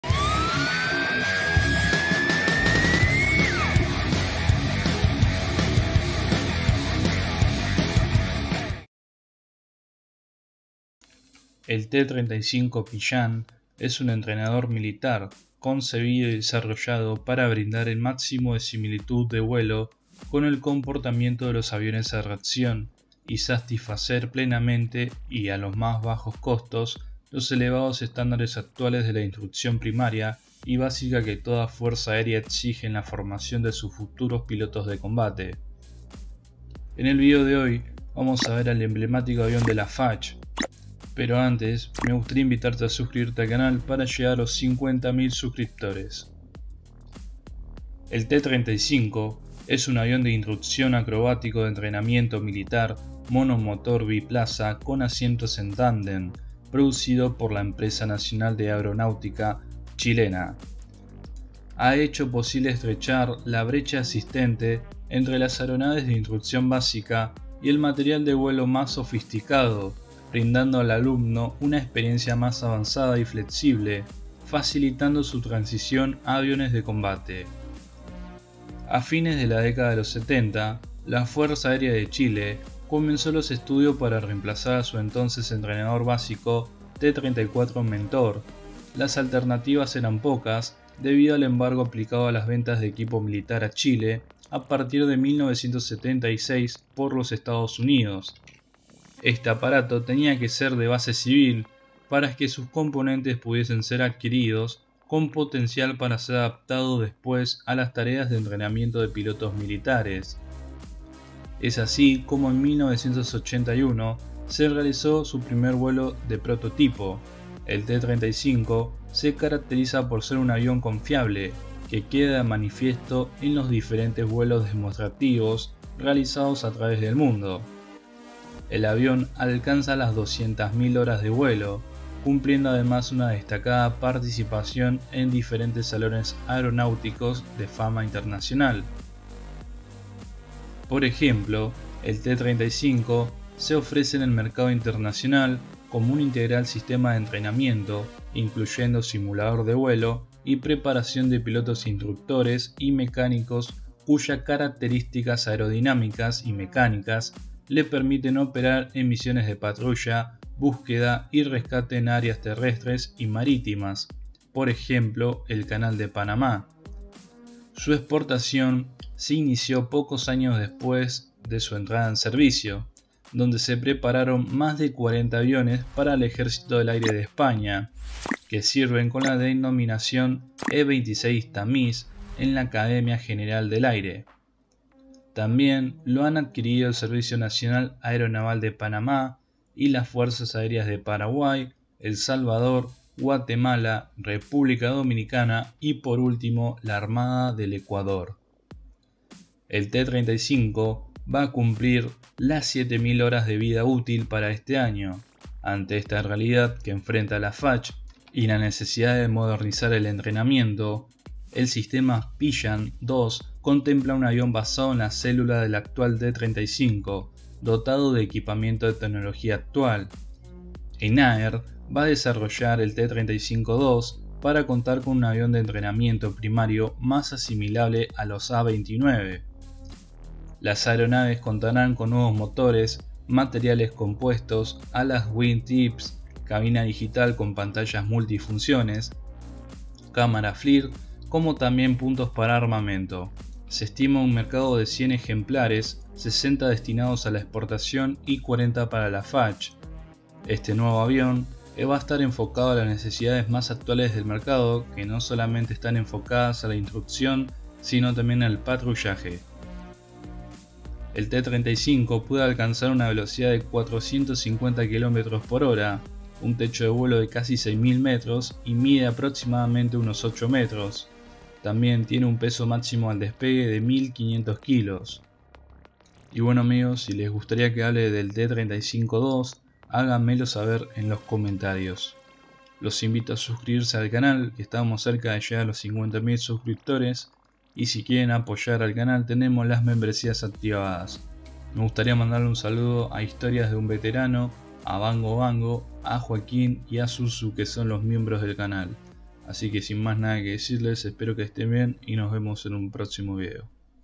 CHILE: El Exitoso Avión "T-35 Pillán" - Hecho en ENAER 🇨🇱
El ENAER T-35 Pillán es un avión de instrucción básica militar fabricado por la empresa chilena ENAER

Consolidado como un avión de instrucción militar, El T-35 es un avión de instrucción primaria, convencional, con asientos tándem, excelente índice de ascenso y con características totalmente acrobáticas, que le permiten gran maniobrabilidad. El Pillán es un avión capaz de despegar con 2900 libras, realizar acrobacias e incluso aterrizar con el mismo peso, sin verse limitado de ninguna forma; lo que lo convierte en el mejor sistema de instrucción y vigilancia. Asimismo, el costo de la hora de vuelo en el Pillán es muy bajo, lo que le permite una operatividad constante en cualquier cielo del mundo.